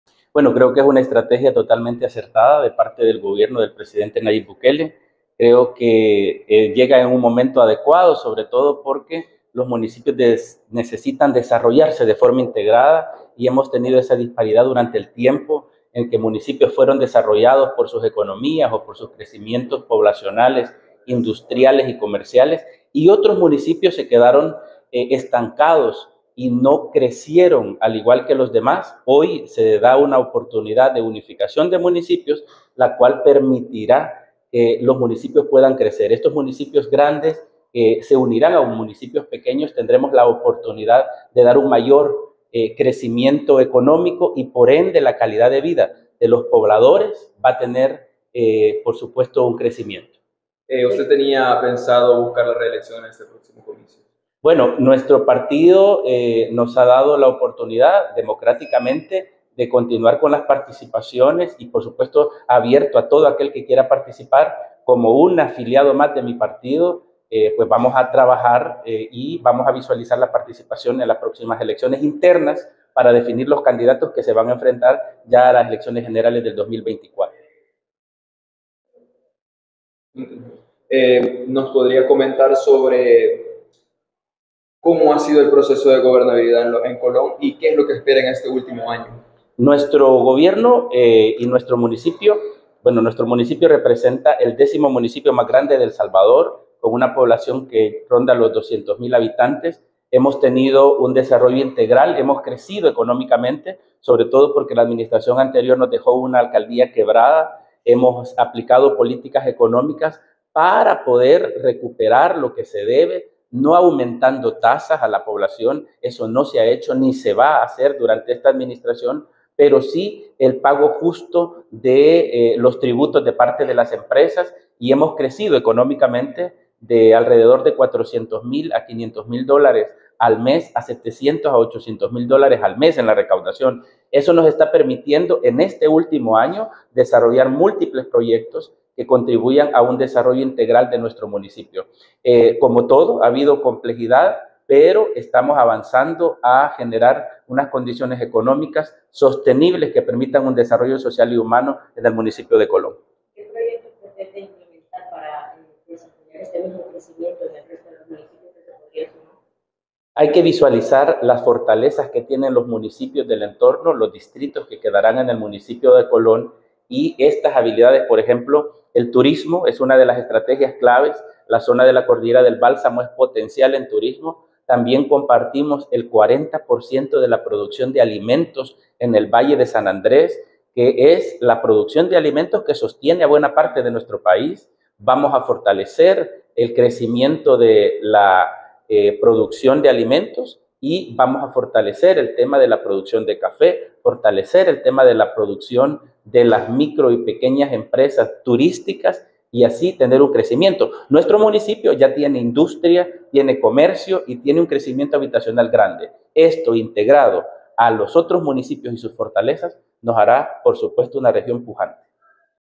"Creo que es una estrategia acertada de parte del Presidente, Nayib Bukele. Llega en un momento"
"Creo que es una estrategia acertada de parte del Presidente, Nayib Bukele. Llega en un momento adecuado porque los municipios necesitan desarrollarse", expuso el Alcalde de Colón, Samael Rivera.